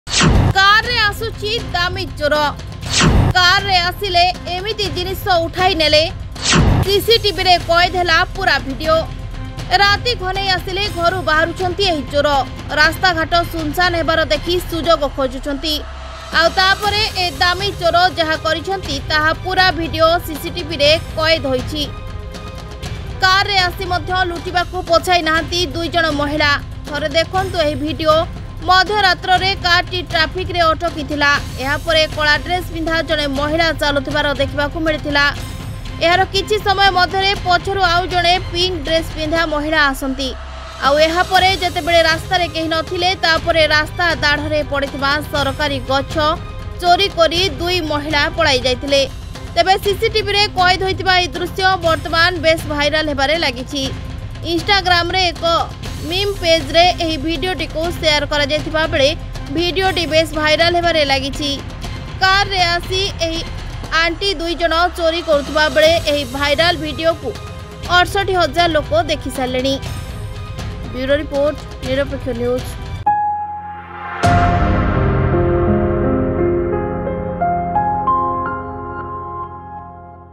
କାରରେ ଆସି ଚୋରୀ କରୁଛି ଚୋର, ସିସିଟିଭିରେ କଏଦ ଭିଡ଼ିଓ robbery | Nirapekshya News
NIRAPEKSHYA NEWS | ODISHA NEWS | ROBBERY COMES IN CAR
ରାତି ଘନେଇ ଆସିଲେ ଘରୁ ବାହାରୁଛନ୍ତି ଏହି ଚୋର। ରାସ୍ତା ଘାଟ ଶୁନଶାନ୍ ହେବାର ଦେଖି ସୁଯୋଗ ଖୋଜୁଛନ୍ତି। ଆଉ ତାପରେ ଏ ଦାମୀ ଚୋର ଯାହା କରିଛନ୍ତି ତାର ପୁରା ଭିଡ଼ିଓ ସିଶିଟିବିରେ କଏଦ ହୋଇଛି । କାରରେ ଆସି ମଧ୍ୟ ଲୁଟିବାକୁ ପଛାଇ ନାହାନ୍ତି ଦୁଇ ଜଣ ମହିଳା । ଥରେ ଦେଖନ୍ତୁ ଏହି ଭିଡ଼ିଓ...
🔆MORE UDATE🔆